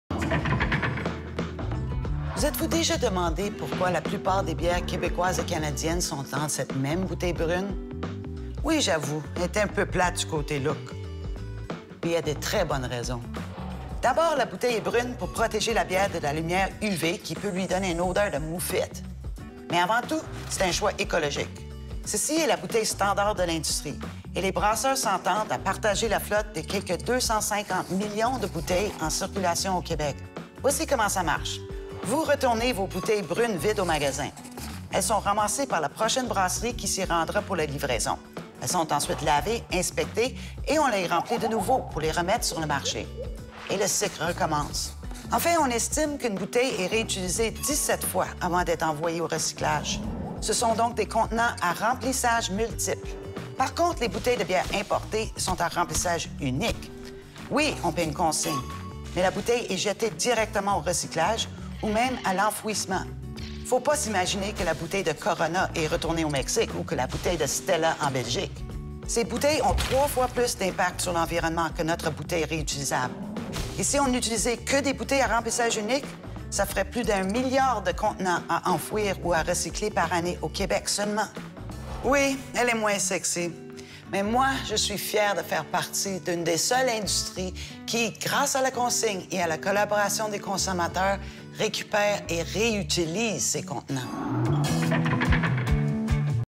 La capsule éducative de Boréale
Pourquoi nos bières Boréale sont dans des bouteilles brunes?